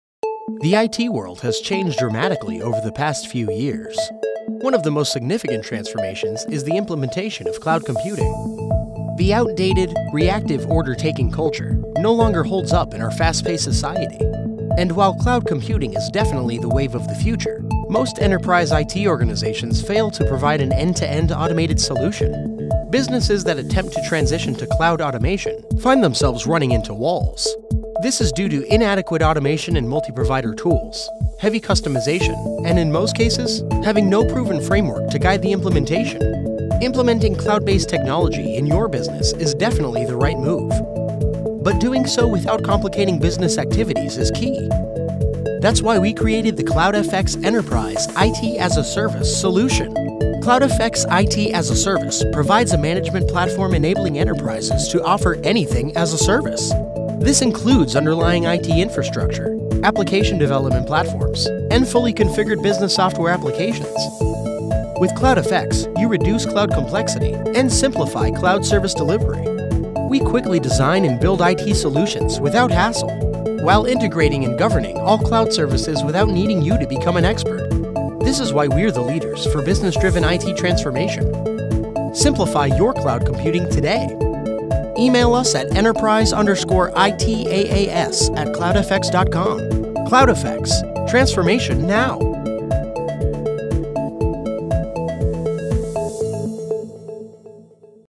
CloudFX - Enterprise ITaaS Solutions
CloudFX has dedicated itself to building the most advanced, functional and flexible Enterprise IT-as-a-Service platform, Hybrid Cloud Services Automation and Management platform and Anything as a Service platform available today.

Our Enterprise Solutions platforms are designed to deliver improved business efficiencies, increased productivity and reduced IT cost and complexity. In parallel, it will provide an underlying technology framework that will enable Enterprises to activate a portfolio of system, connectivity and cloud orientated services and begin to effectively leverage innovative business service capability through one automated and managed platform enabling multi-vendor Infrastructure, Platform and Application services across your business in a secure and scalable model.